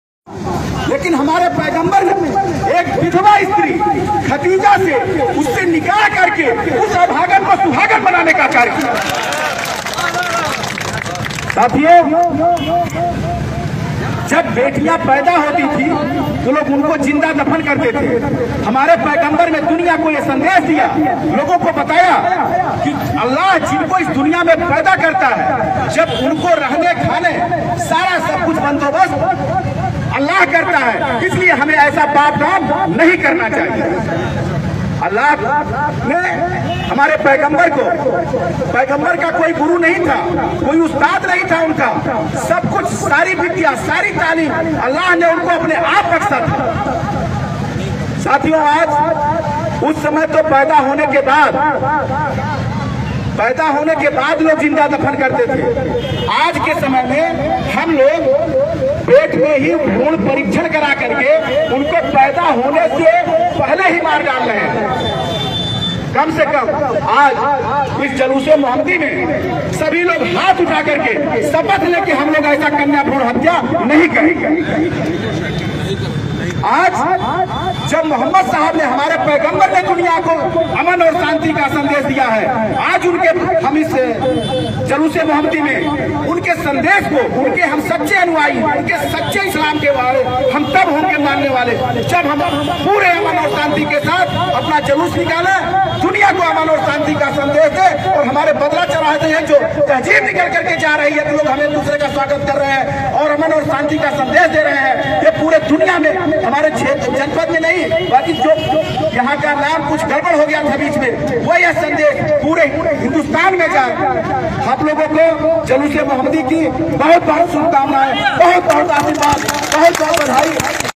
ZARknowledgechanel Milad un-Nabi/Id-e-Milad | UP Police ne Hazrat Muhammad S.A.W. Tarif ki
Milad un-Nabi/Id-e-Milad | UP Police ne Hazrat Muhammad S.A.W. Tarif ki aap Hazrat Mohammed Sallallahu Alaihi vasllam ke Umr 25 sal ki thi aapane Hazrat Khadija se Nikah Kiya use Waqt Paigambar Muhammad Ki Umr 25 sal ki thi aur Khadija ki Umr 40 sal ki thi